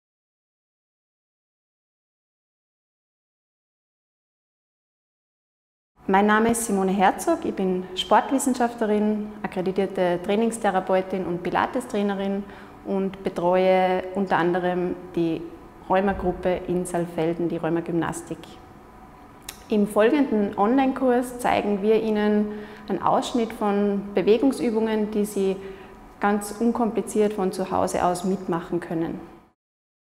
Bewegungsübungen bei rheumatoider Arthritis | Patientenschulung
Regelmäßige Bewegung kann nachweislich Beschwerden lindern, die durch entzündlich-rheumatische Erkrankungen hervorgerufen werden. Deshalb sollten PatientInnen mit rheumatoider Arthritis ausreichend Bewegungsübungen und Trainingseinheiten in ihren Alltag integrieren.
In diesem Kurs zeigt Simone Herzog Ihnen gemeinsam mit der Rheumagruppe Saalfelden eine Vielzahl von Bewegungsübungen, die bei rheumatoider Arthritis geeignet sind und die Ihnen dabei helfen, in Bewegung zu bleiben.

» HILFREICHE VIDEOS ZU RHEUMATOIDER ARTHRITIS

» KOSTENLOSE DOWNLOADS ZU RHEUMATOIDER ARTHRITIS

➡️ Hol Dir den kostenlosen selpers-Newsletter mit vielen Tipps rund um deine Erkrankung